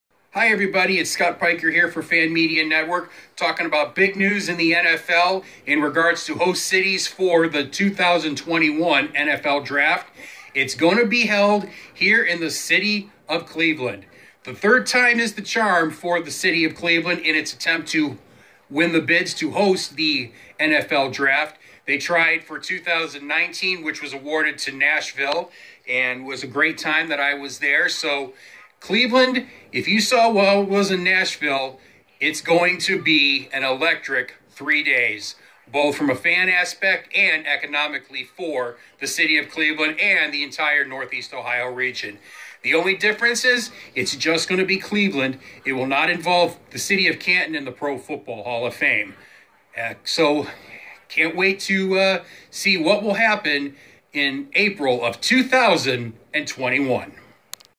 NFL Awards 2021 NFL Draft to Cleveland - NFL Fan Media Video
NFL news video made on a mobile phone. Fan Media Network – Sports News.